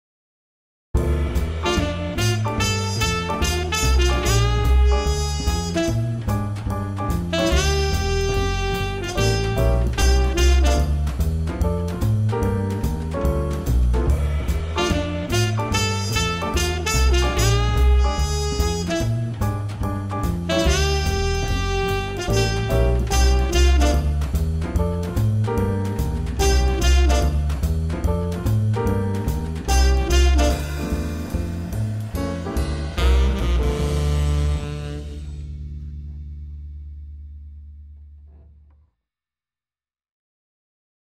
右折禁 危険運転者 松本市 渚 国道１９号線
看板も標識も意味が解らない様です。